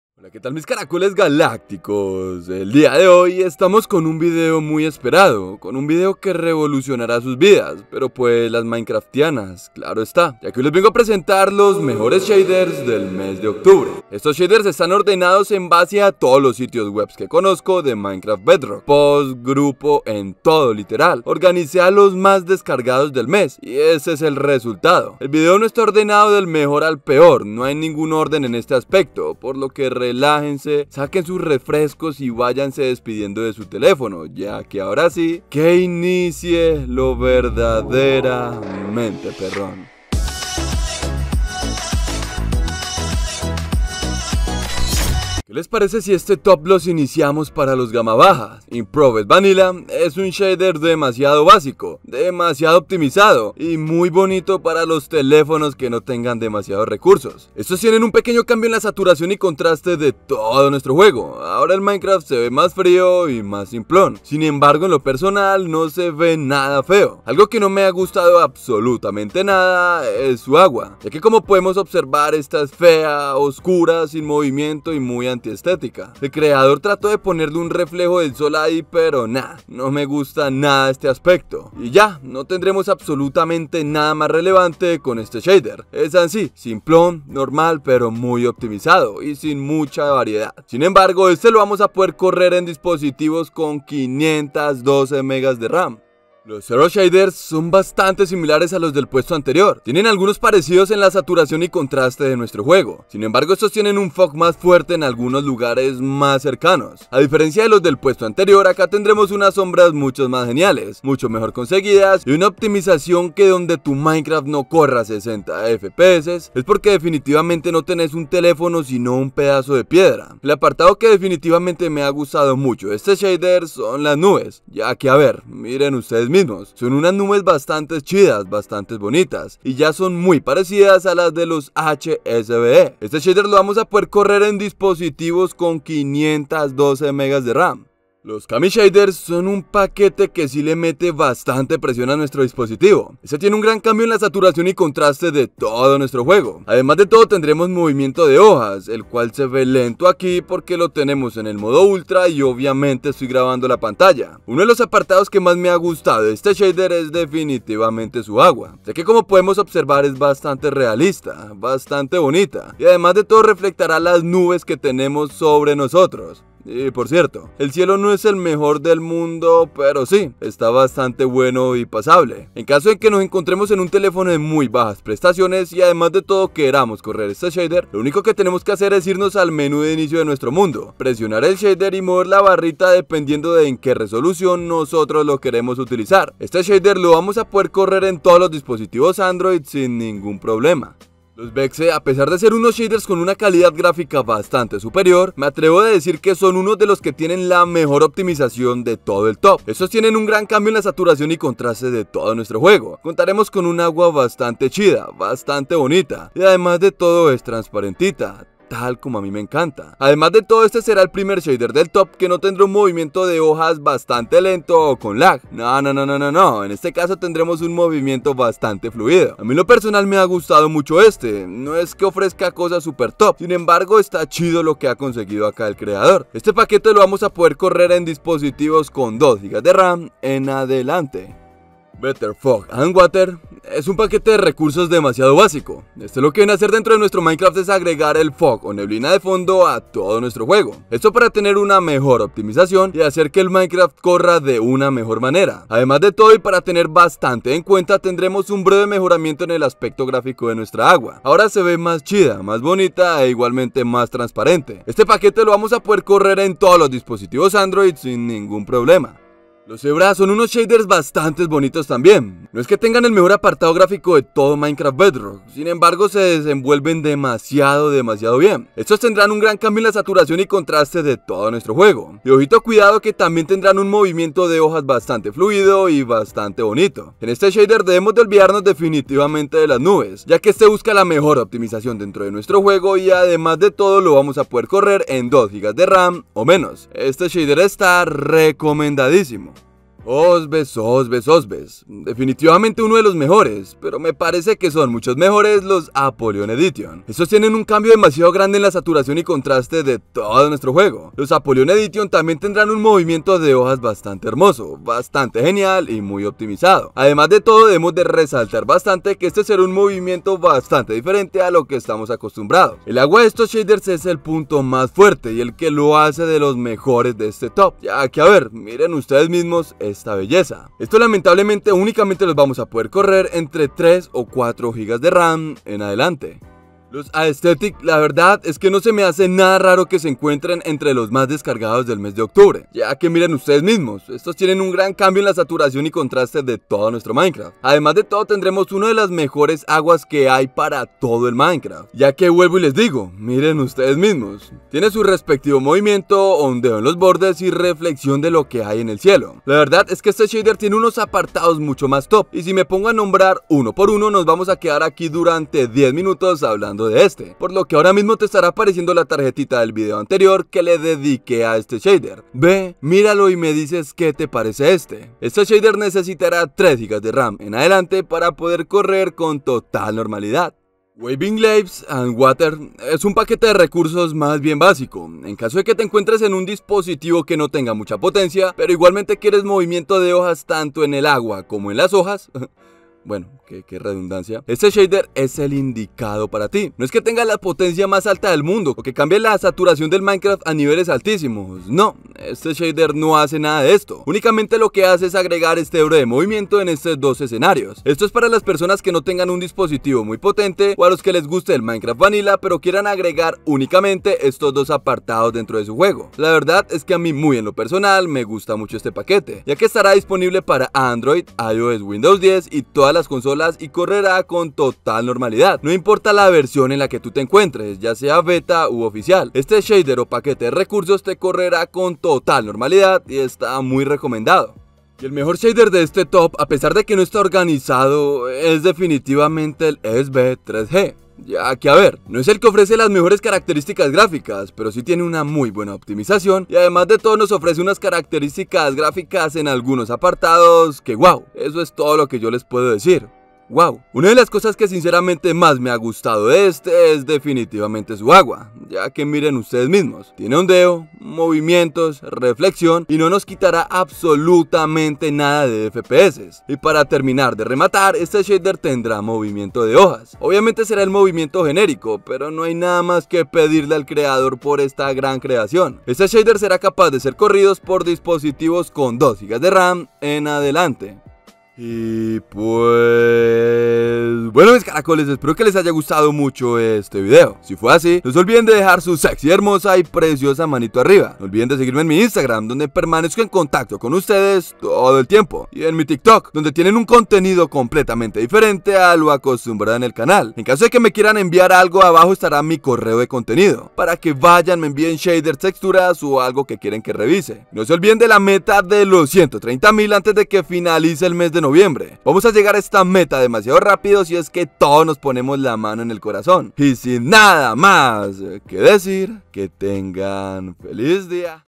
Top 10 Los Mejores Shaders del Mes (Gama Baja, Media, Alta)
🌌En caso de que estuvieras en busqueda de un TOP CON LOS MEJORES SHADERS DEL MES, estas en el lugar indicado! Hoy hay shaders de todos los olores, sabores y colores... Por lo que te recomiendo que mires el video entero y descargues los que más te gusten!

🔔TimeStamps🔔
0:00 Intro
0:45 IVPE
1:36 Zero
2:19 Kamii
3:11 VexZe
3:52 Better Fog And Water
4:19 Zebra
4:50 Osbes
5:26 Aesthetic Fantasy
6:11 Waving Leaves And Water
7:05 Esbe 3G